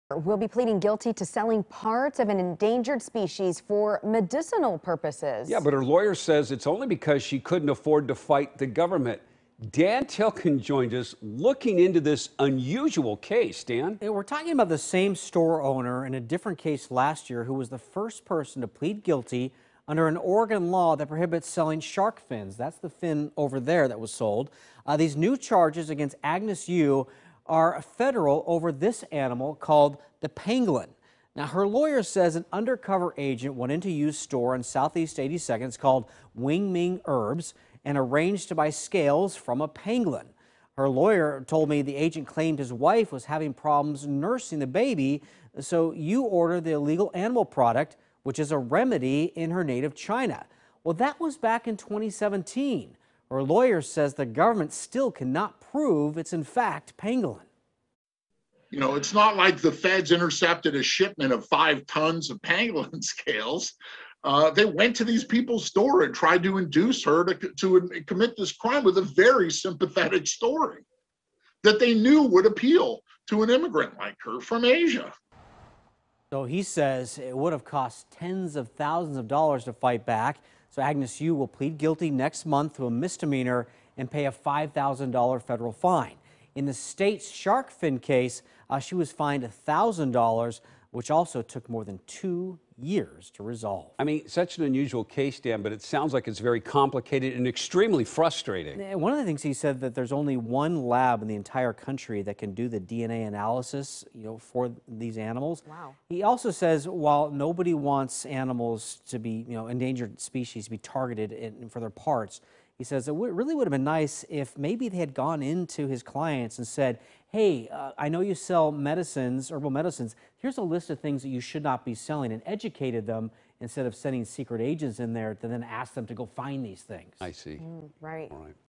Selling pangolin scales: Lawyer says client had material as remedy
A Happy Valley woman previously charged with illegally selling shark fins is now accused of having sold pangolin scales.